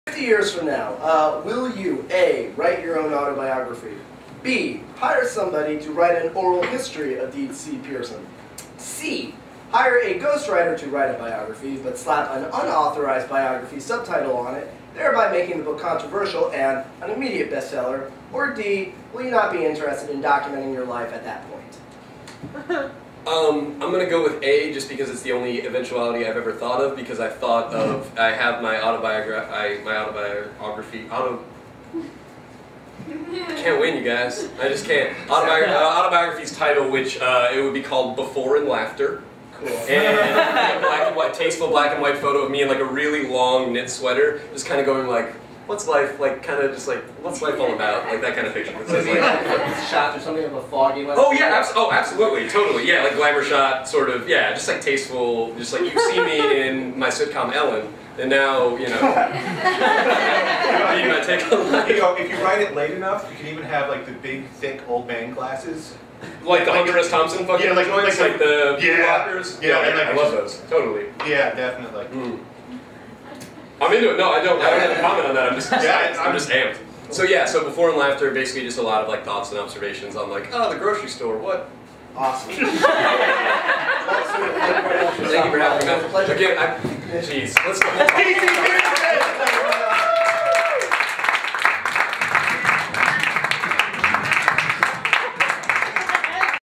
The Matt Fried Hour: The DC Pierson Autobiography
DC Pierson talks about his eventual autobiography. Live at Under St. Marks Theater, 2/21/09.